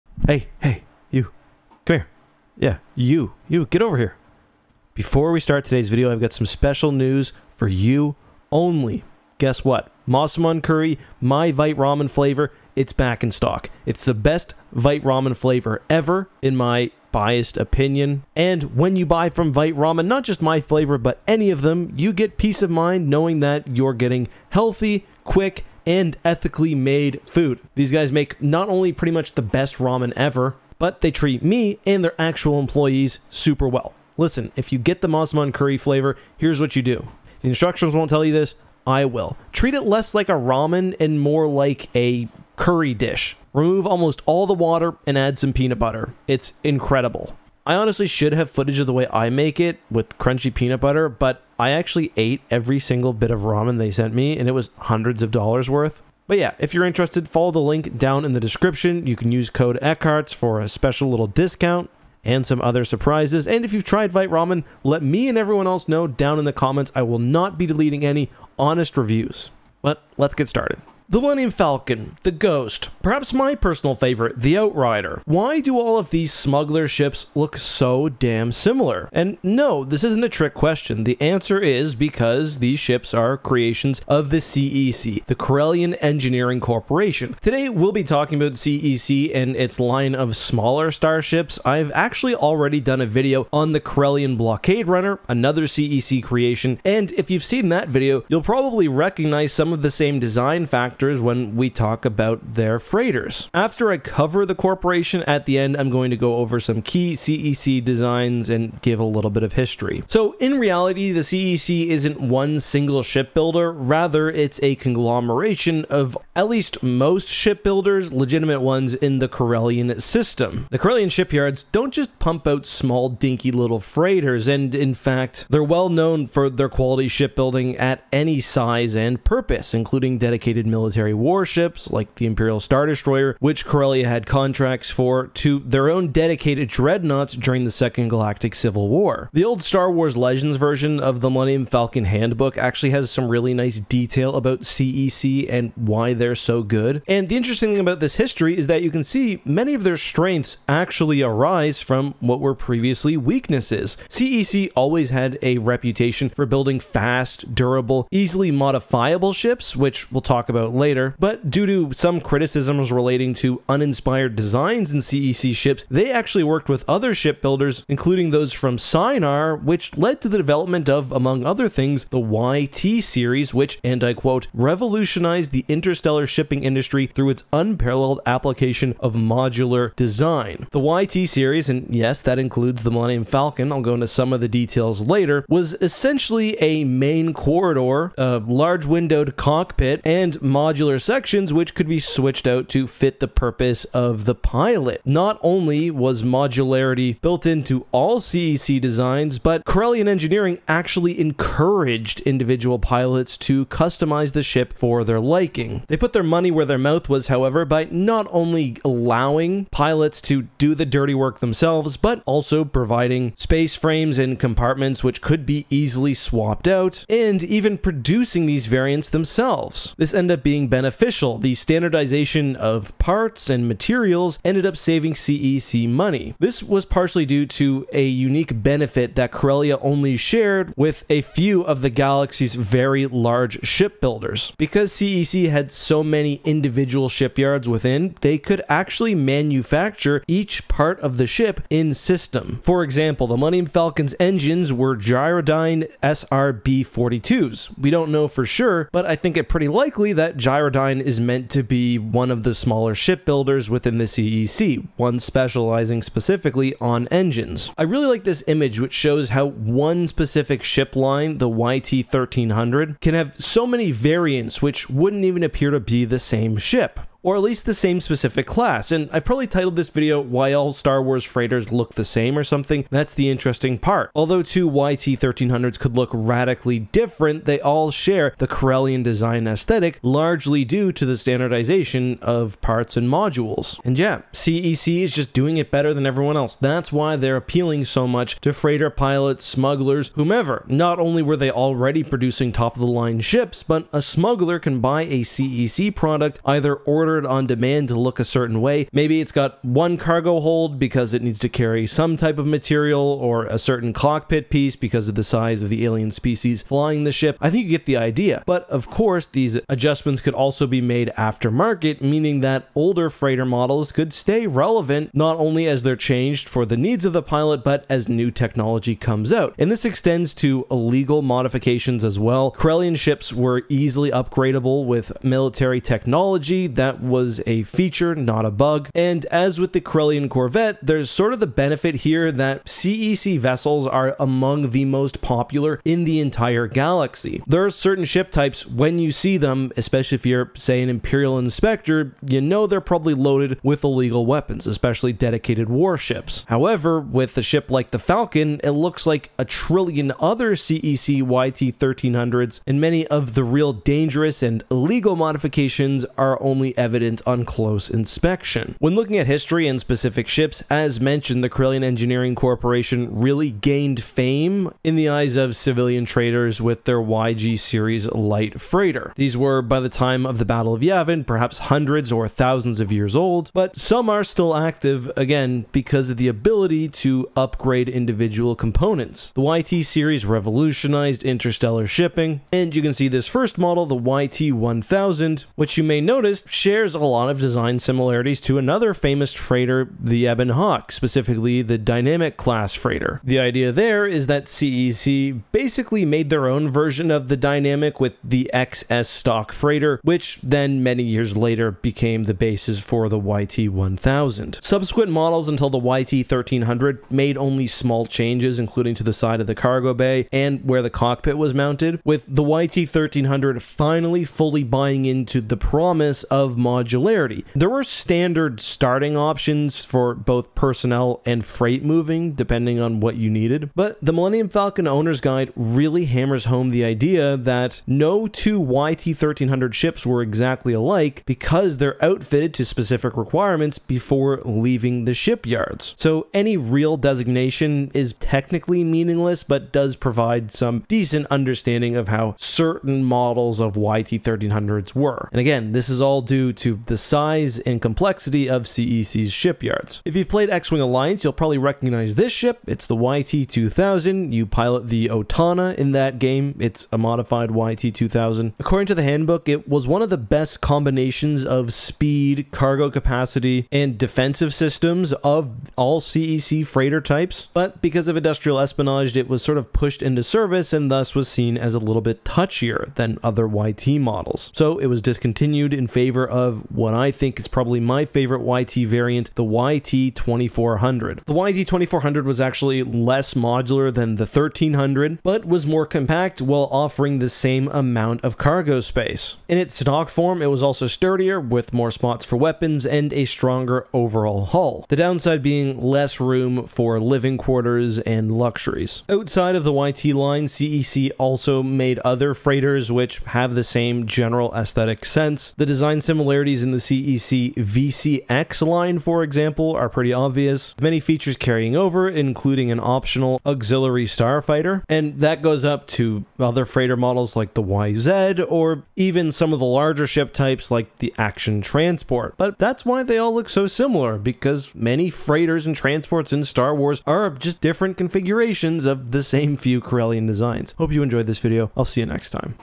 Why Star Wars Freighters always look the same
Today I want to take a closer look at freighters within the Star Wars universe, with a focus on the Corellian Engineering corporation. We'll talk about all that and more on today's Star Wars Legends Lore video.